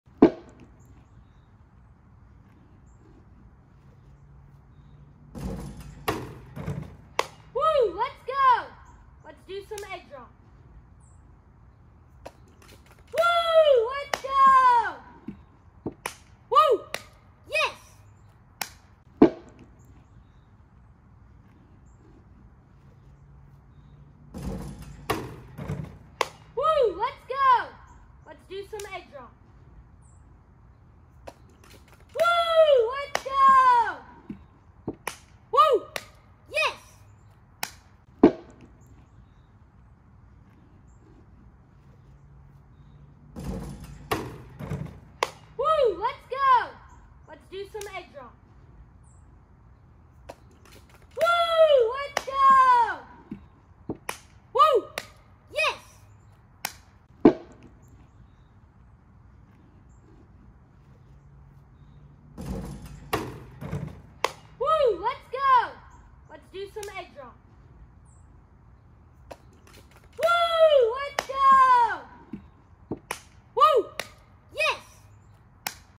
10-Year-Old Pulls Off Amazing Basketball Trick Shot
A skilled 10-year-old from Augusta, Georgia, shows off an impressive basketball trick shot with three basketball hoops while standing on a balance board, video filmed on December 7 shows.
Credit: Jaclyn Vozniak via Storyful